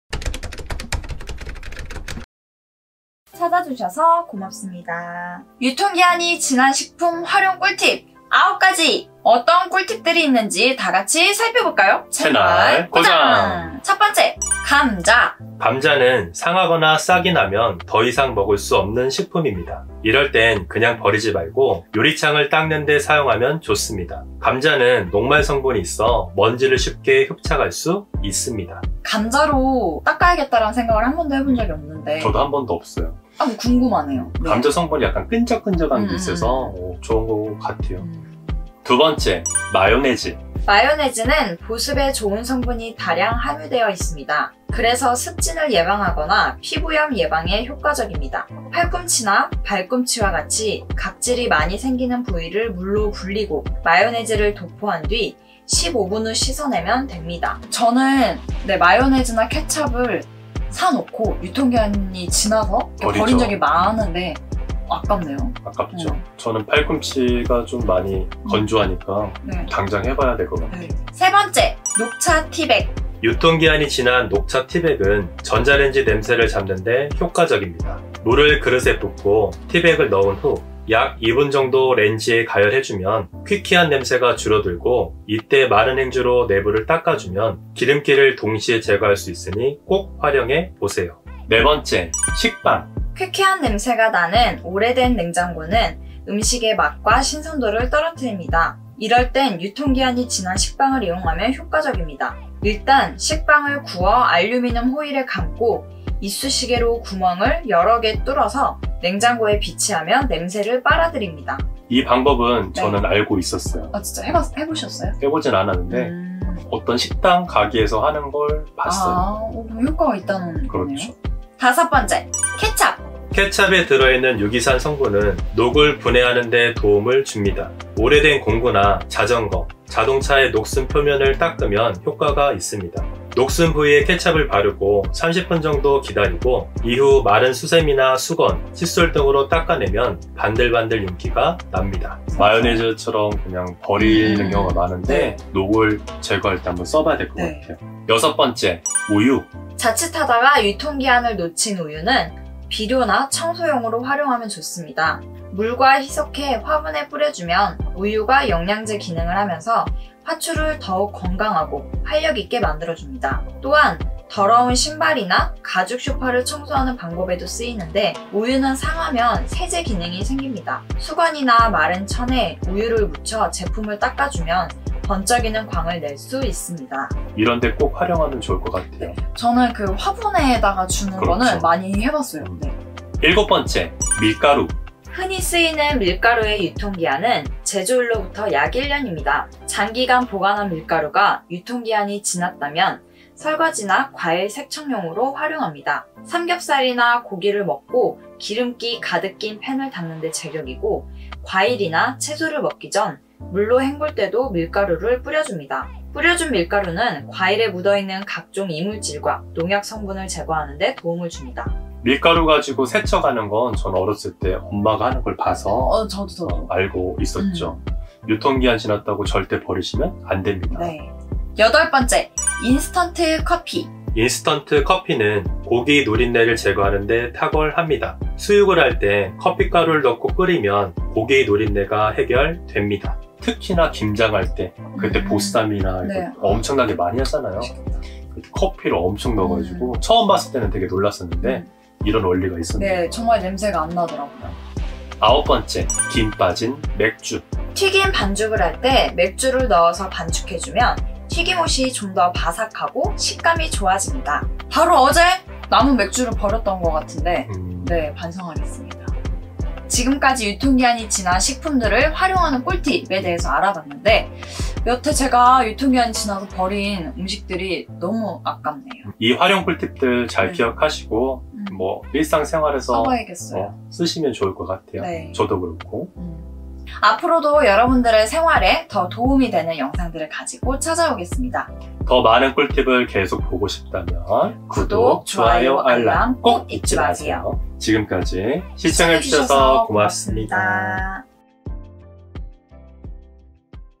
유통기한 지난 식품 활용 꿀팁 9가지
유통기한 지난 식품 유통기한 지난 우유 유통기한 지난 음식 활용법

기한이 지났다고 버리지 마세요!!!
다시 활용할 수 있는 식품들이 어떤것들이 있는지 확인하세요!

꼭!